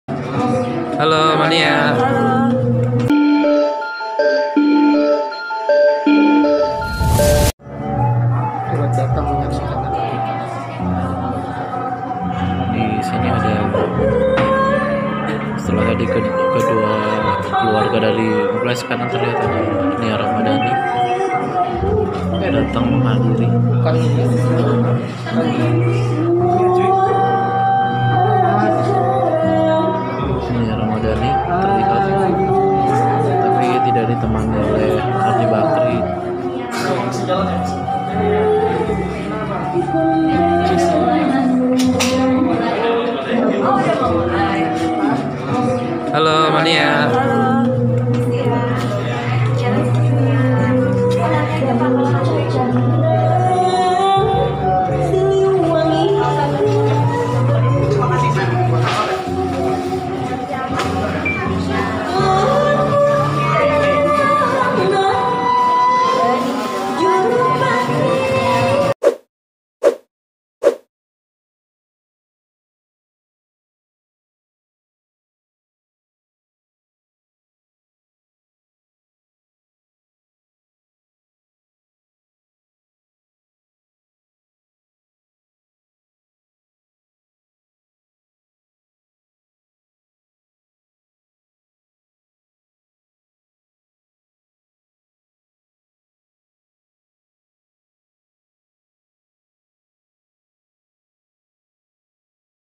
Nia Ramadhani Ada di Akad Nikah Rezky Aditya dan Citra Kirana
Jelang akad nikah Rezky Aditya dan Citra Kirana, Minggu (1/12) di sebuah hotel kawasan Bandung, Jawa Barat, terlihat Nia Ramadhani datang dengan langkah cepat. (Gede/Beepdo.com)